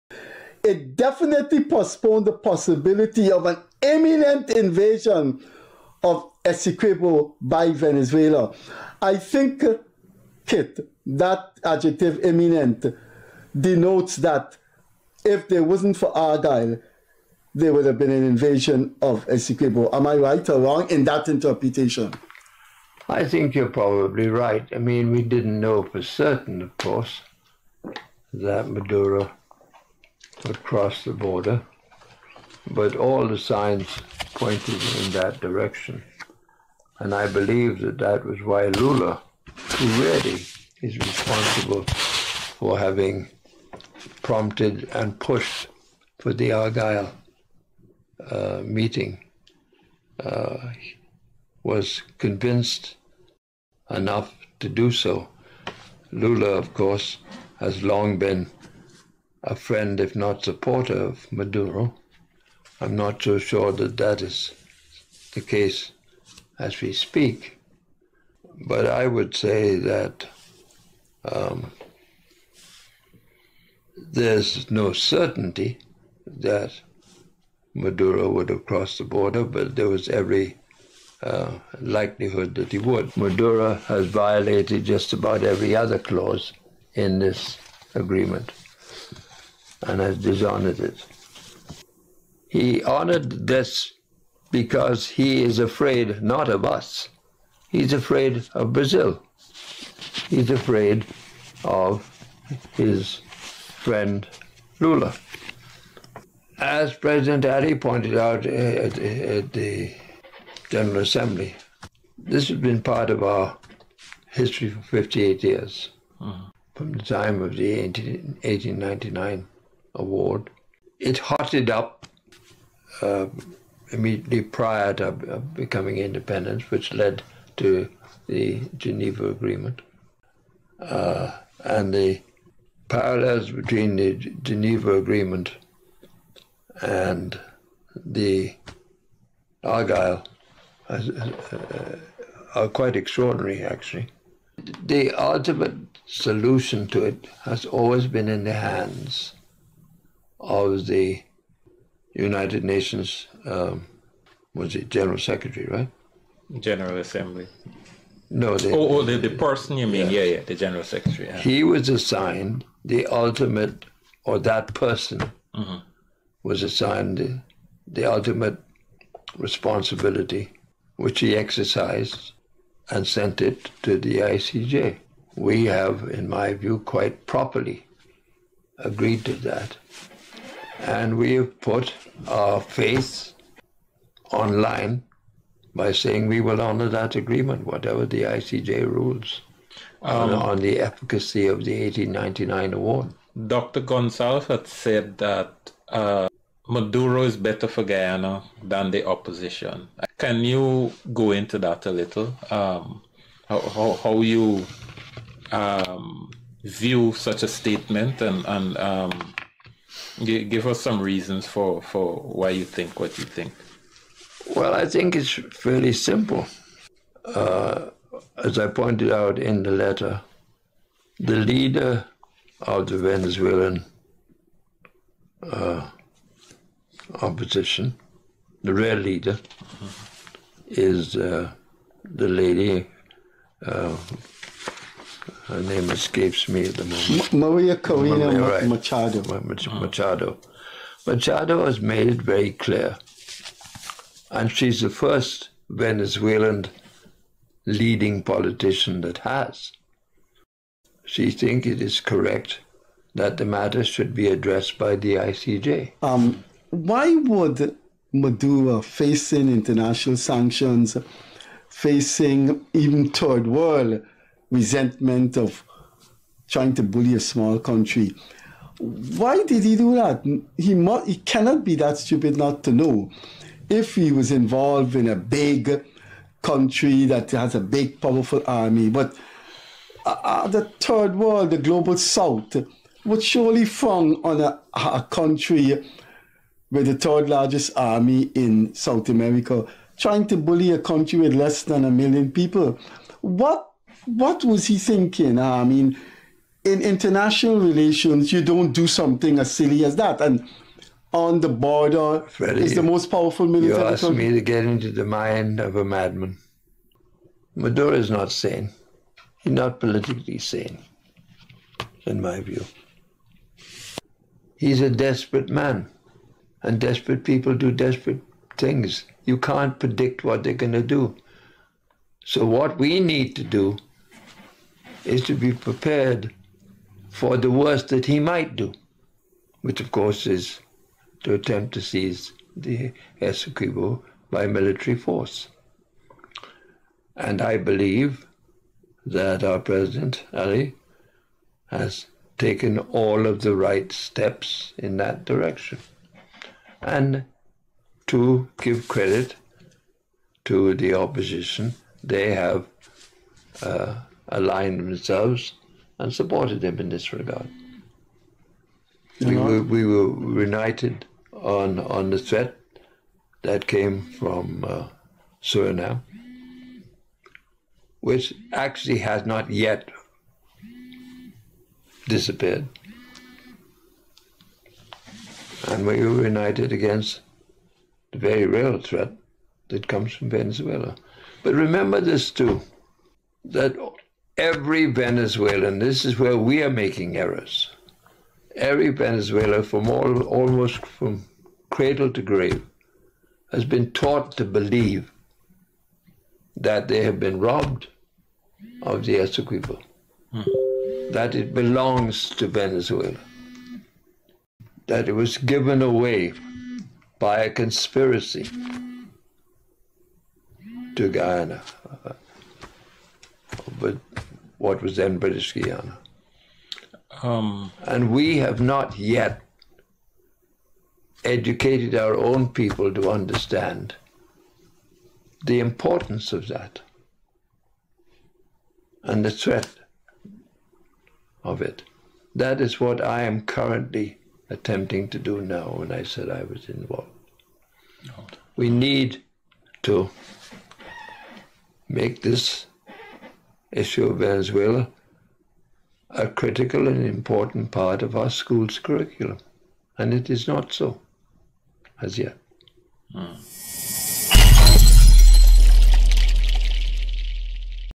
"Guyana debería estar lista para una invasión venezolana" Kit Nascimento
Kit Nascimento, Asesor de Comunicaciones de Irfaan Ali, asegura que sin el Tratado de Argyles, Venezuela ya hubiese avanzado militarmente en el Esequibo

Igualmente señala que el Presidente Maduro ha violado dicho Tratado. Maneja la mediática en confiar en la Corte Internacional de Justicia.

Respalda y aplaude el camino de Maria Corina Machado en llevar el caso a la CIJ. Igualmente continúan con la mediática de ser un país pequeño acosado por la tercera potencia militar de Suramérica.

Dice que Guyana debería estar lista para una confrontación militar. Recordó cuando Guyana se unió en contra del "acoso por parte de Surinam, el cual aún no ha terminado".

Indica que, al contrario de Venezuela, quienes han educado a los escolares sobre el esequibo, Guyana no implementa ello en los pensum de las escuelas.